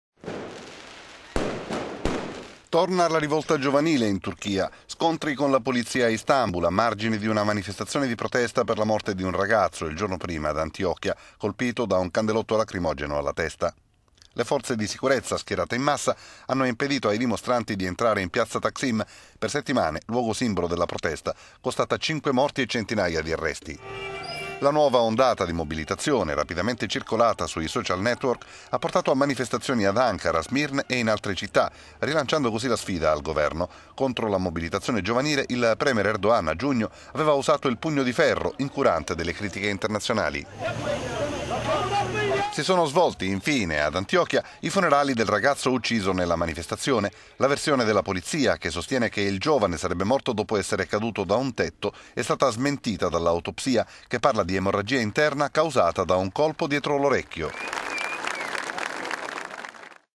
Turchia, torna la protesta dopo la morte di un manifestante ad Antiochia
Torna la rivolta giovanile in Turchia. Scontri con la polizia a Istanbul, a margine di una...

Torna la rivolta giovanile in Turchia. Scontri con la polizia a Istanbul, a margine di una manifestazione di protesta per la morte di un ragazzo il giorno prima ad Antiochia, colpito da un candelotto lacrimogeno alla testa.

Le forze di sicurezza, schierate in massa, hanno impedito ai dimostranti di entrare in Piazza Taksim, per settimane luogo simbolo della protesta costata cinque morti e centinaia di arresti.

La nuova ondata di mobilitazione, rapidamente circolata sui social network, ha portato a manifestazioni ad Ankara, Smirne e altre città, rilanciando così la sfida al governo. Contro la mobilitazione giovanile il premier Erdogan, a giugno, aveva usato il pugno di ferro, incurante delle critiche internazionali.

Infine, si sono svolti i funerali del ragazzo ucciso nella manifestazione. La versione della polizia - che sostiene che il giovane sarebbe morto dopo essere caduto da un tetto - è stata smentita dall'autopsia, che parla di "emorragia interna causata da un colpo dietro l'orecchio".